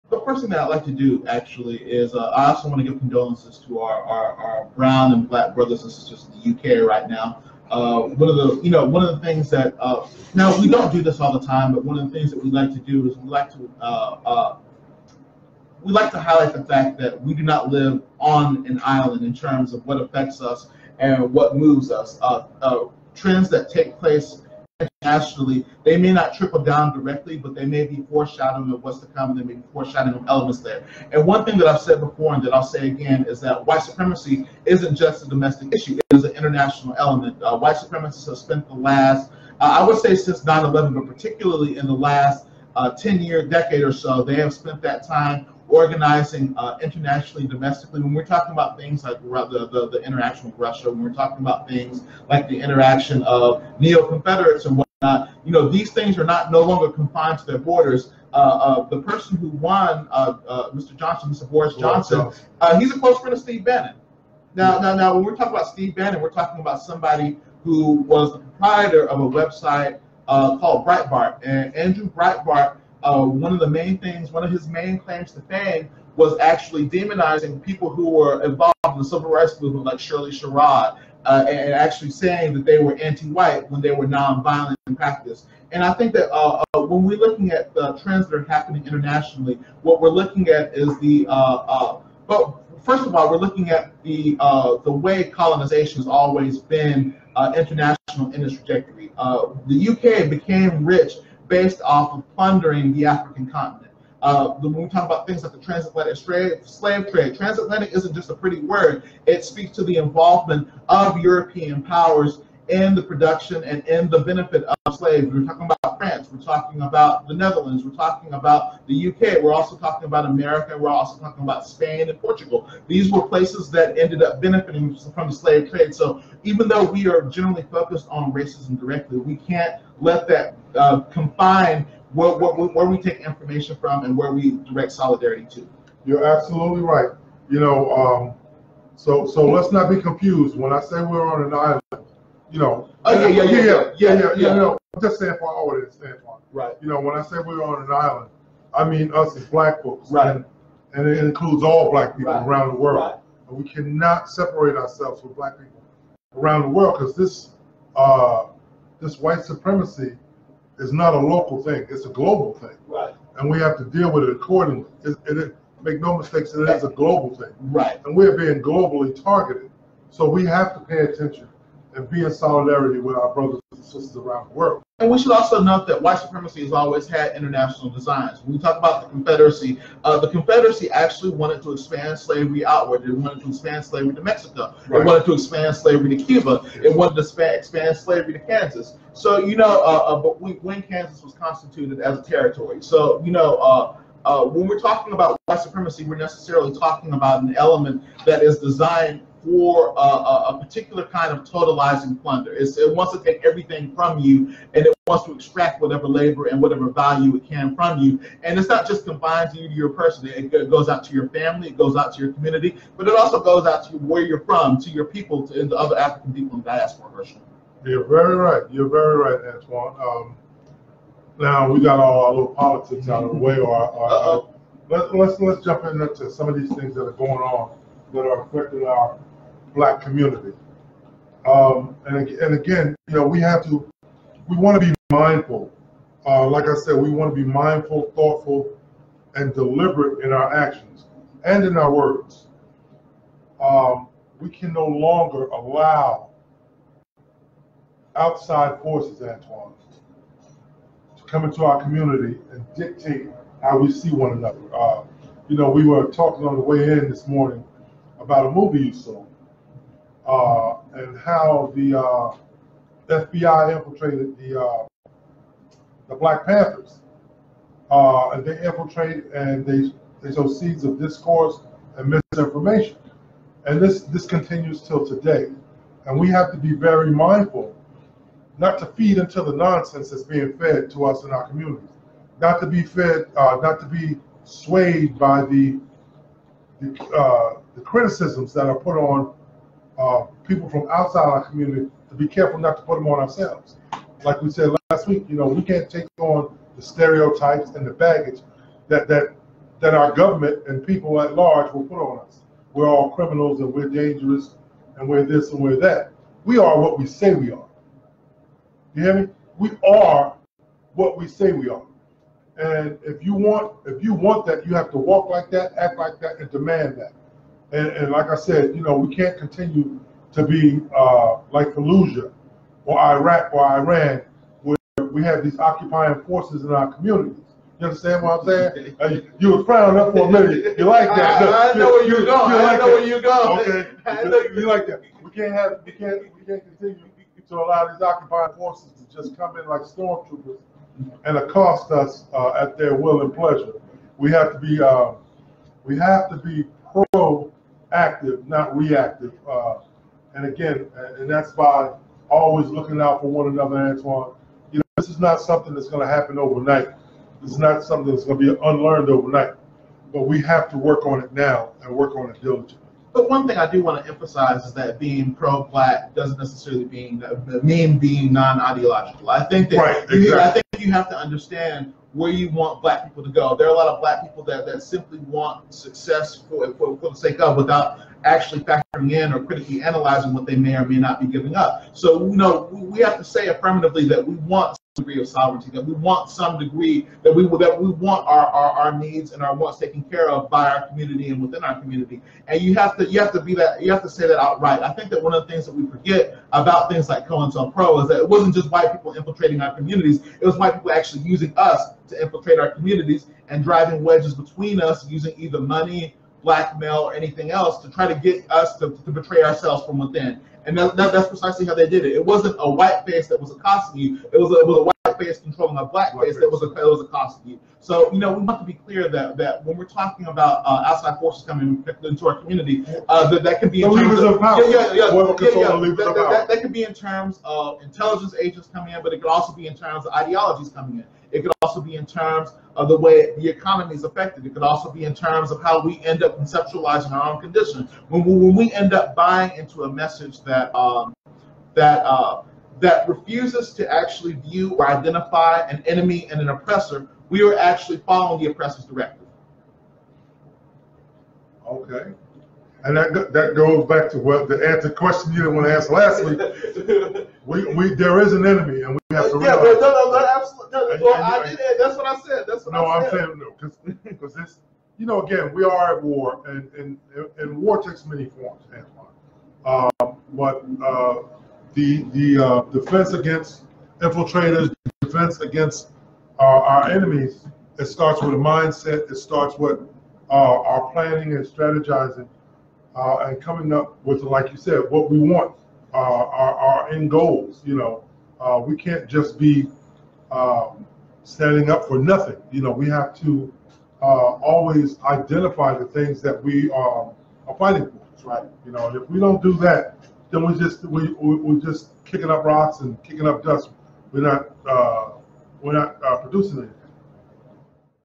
Boris Johnson, Breitbart and White Supremacy As A Global Phenomenon
All power to the people!